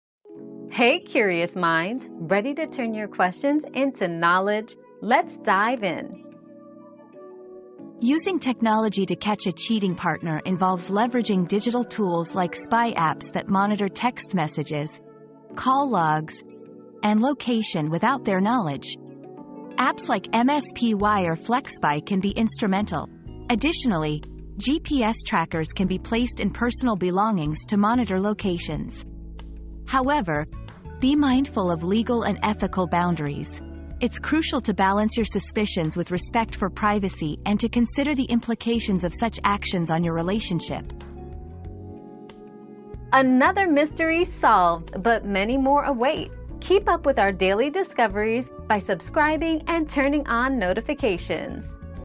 How to use technology to catch a cheating partner?
Digital Detective: Catching a Cheating Partner with Tech 👉 Tech Sleuthing 👉 Learn how to use spy apps and GPS trackers to uncover infidelity discreetly. But remember to tread carefully to protect privacy and relationship dynamics.

Laura S. Harris (2024, May 6.) How to use technology to catch a cheating partner?
    🌐 AskAbout.video